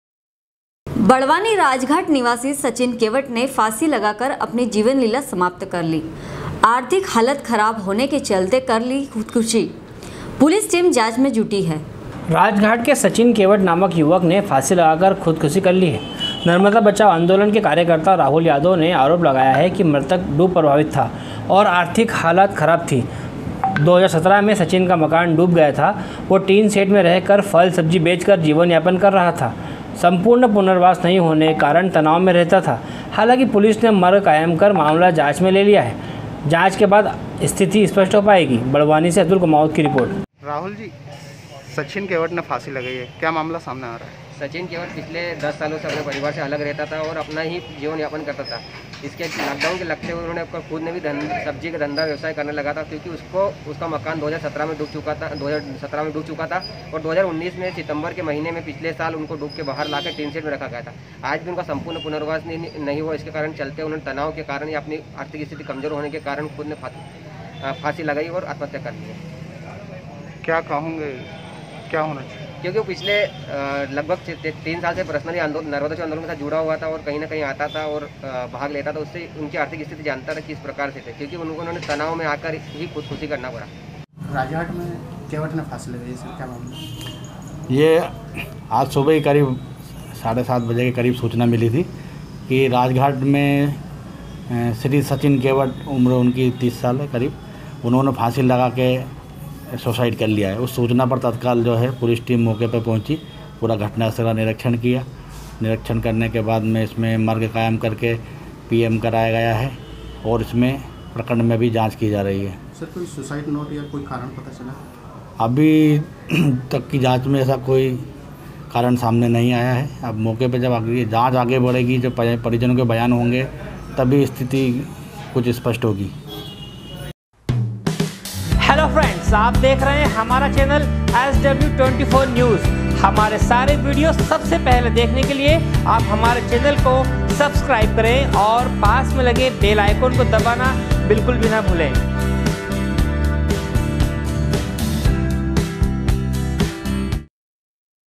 SW 24 News Madhyapradesh
बड़वानी राजघाट निवासी सचिन केवट ने फाँसी लगाकर अपनी जिवन लिला समाप्त कर ली आर्थिक हालत खराब होंने के चलते कर ली खुदखुशी पुलिस टिम जांच में जुटी है।
 बड़वानी से अतुल कुमावत की रिर्पोट